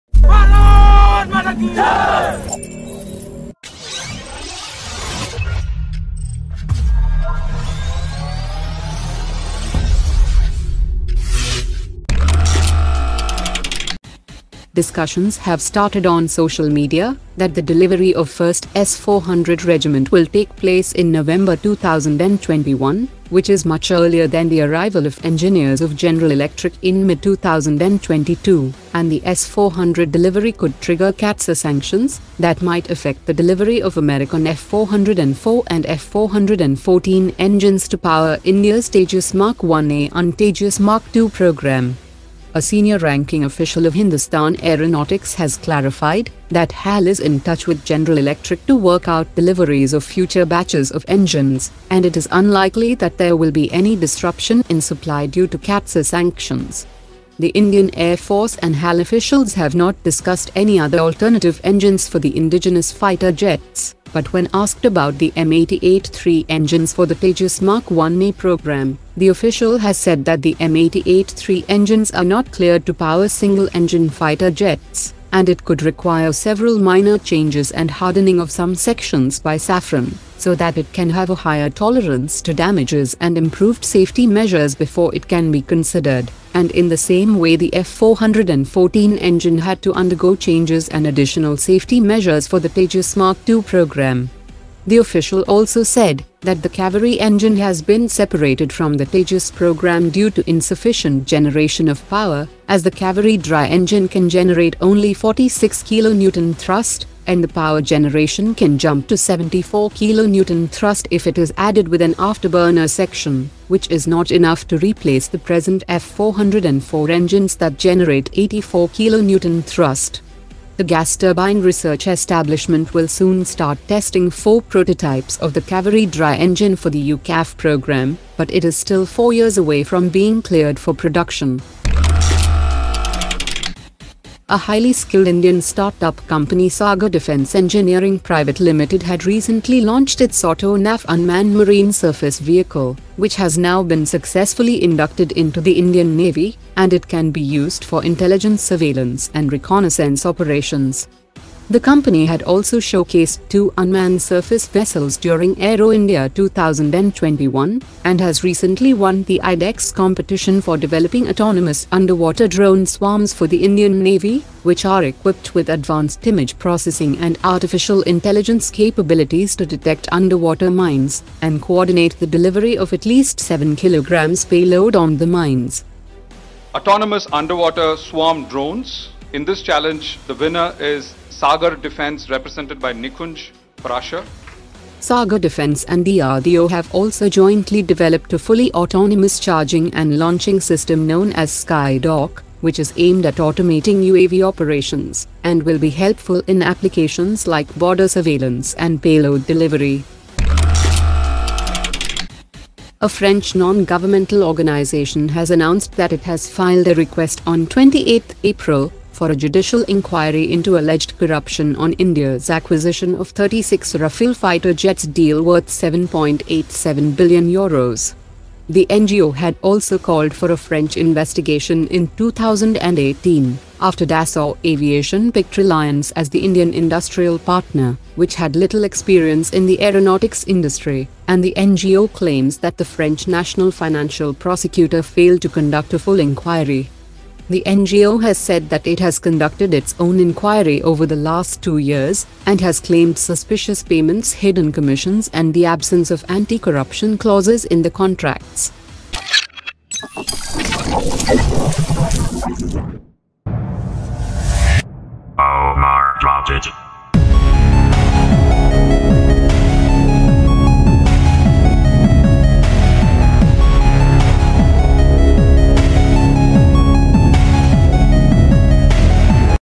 Indian Defence Updates : CAATSA on Tejas Mark2,Under Swarm Drones,UCAV Engine Testing,Rafale Enquiry
1. Unlikely to face sanctions on US engines for TejasMark2 : Kaveri Engine Delinked from Tejas program : No Safran M-88-3 alternative for Tejas : Dry Kaveri engine 4 prototypes testing soon for UCAV : Top HAL Official.
2. Sagar Defence developing 'UnderwaterSwarmDrones' for Indian Navy - DRDO Sagar Developed Skydock, Unmanned Surface Vessels.
3. French NGO Sherpa pursues inquiry into 36 Rafale acquisition.